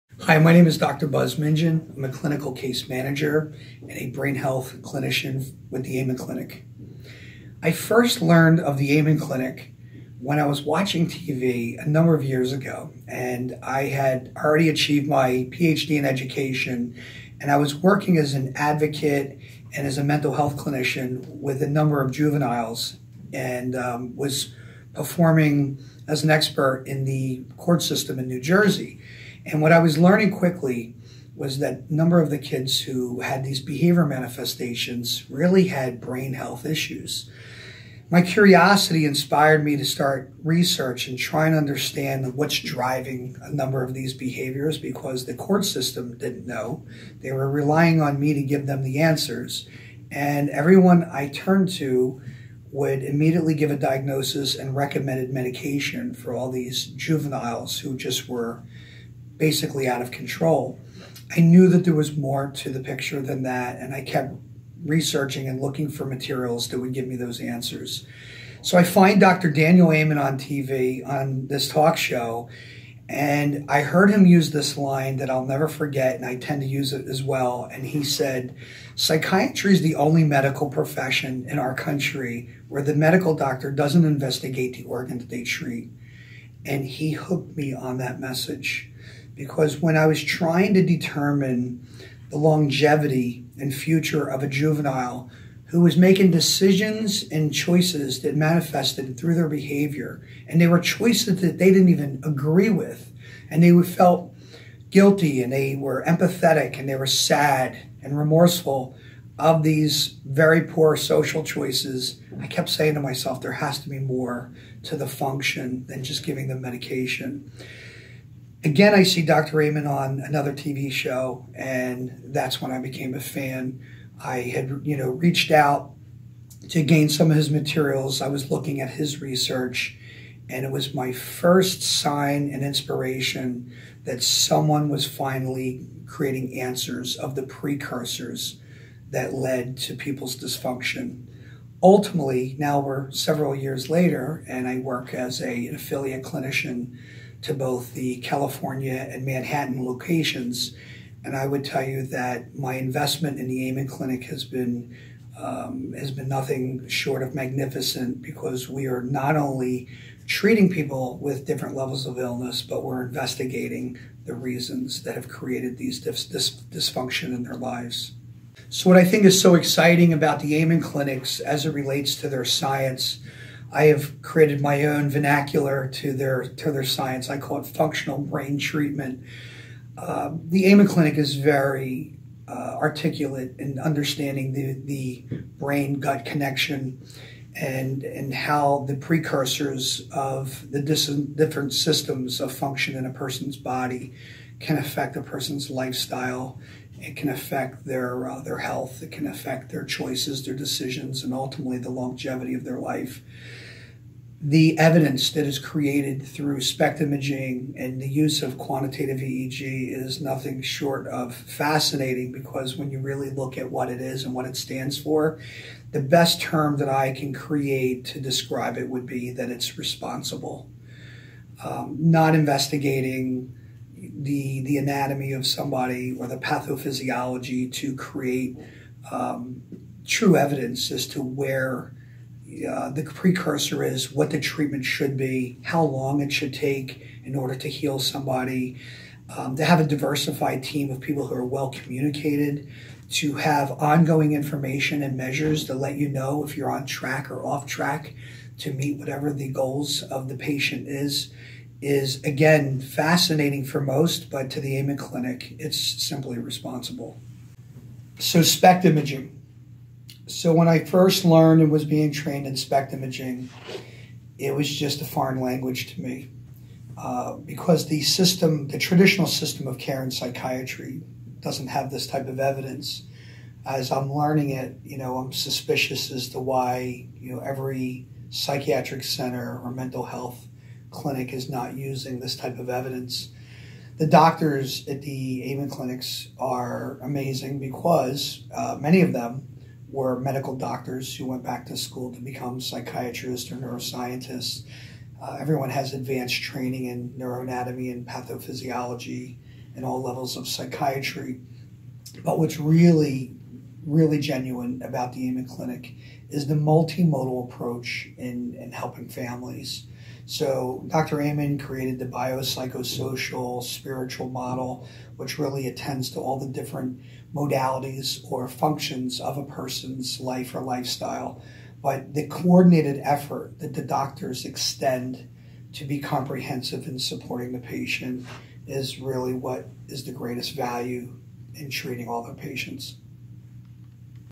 Dr. Buzz Mingin on Why He Became an Amen Clinics Affiliate Clinician
Many people know Dr. Buzz Mingin for his work with pop star Justin Bieber, but he first became interested in looking at the brain when he saw Dr. Daniel Amen on TV. In this video, Dr. Mingin discusses his path to "functional brain treatment" and how it led to a break from the traditional psychiatric model of prescribing medications without looking at the brain. Mingin makes a clear case for why we need SPECT and QEEG imaging in order to make responsible psychiatric decisions.